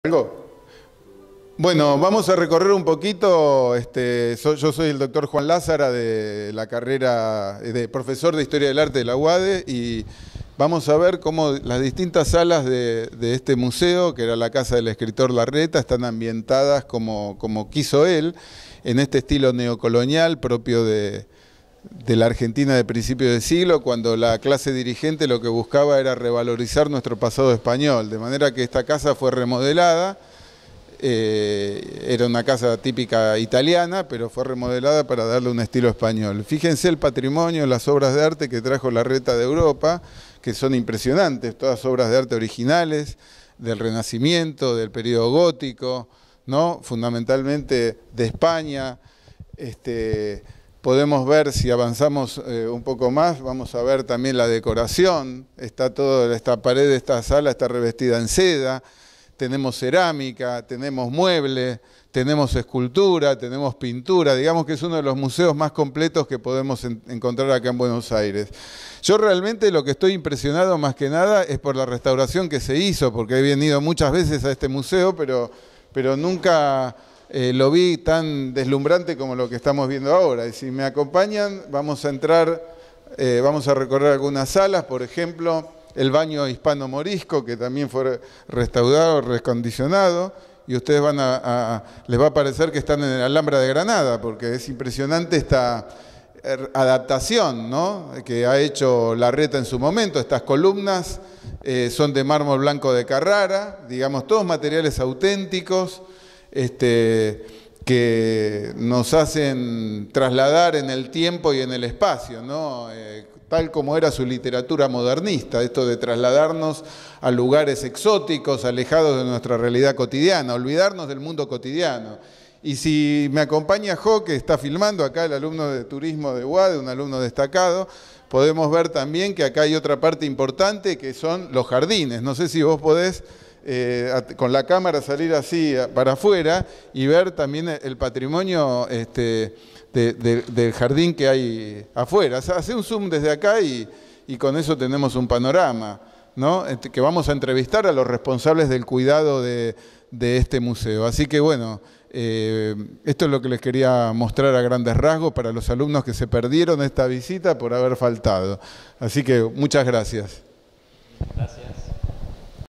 DR JUAN LAZARA RECORRE MUSEO LARRETA JUNTO AL ALUMNO DE UADE EMILIANO HO
El Dr. Juan Antonio Lázara realizó una visita al Museo Larreta  con sus alumnos de la licenciatura en Turismo y Hotelería de la UADE. Los resultados de la visita quedan de manifiesto en esta lista de reproducción que invitamos a visualizar. El Museo Larreta no tiene parangón y es el mejor museo de Buenos Aires. Gracias a las autoridades que nos recibieron, la directora Lic. Delfina Helguera, su coordinador de educación, Marcelo Ayuso, su jefe de jardineros Antonio Sturla, sus colaboradores Matías y Martín y a la Lic. Cecilia de la Fuente nuestra destacada ex alumna de Patrimonio Público II de ESEADE y hoy con una inserción laboral merecida en el Larreta. No dejen de visitar el Museo de Arte Español Enrique Larreta y acerquen sus comentarios. Dr. Juan Antonio Lázara